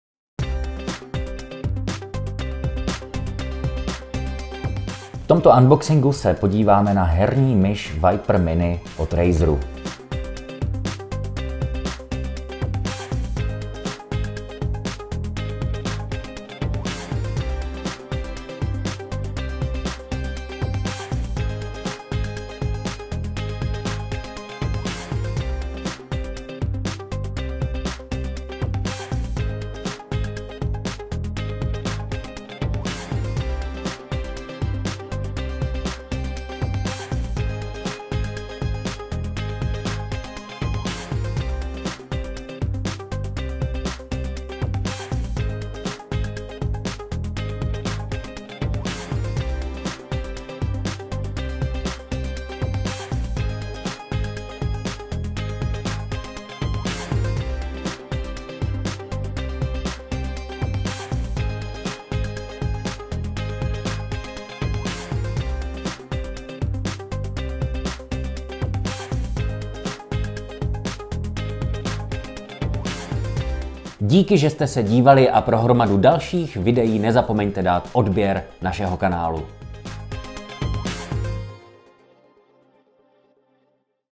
Myš Razer Viper Mini černá | unboxing
Razer navrhl herní myš Viper Mini tak, aby byla kompaktní (ideální do menších rukou) a přitom poskytla prémiovou rychlost, přesnost i životnost stejně jako její větší sestry.Na myši Razer spolupracoval s esportovci, aby dosáhl ideálního designu a absolutní kontroly nad ovládáním. Výsledkem je například, že nabídne špičkový optický senzor s DPI až 8 500 nebo tlačítka reagující v neuvěřitelném tempu už za 0,2 ms.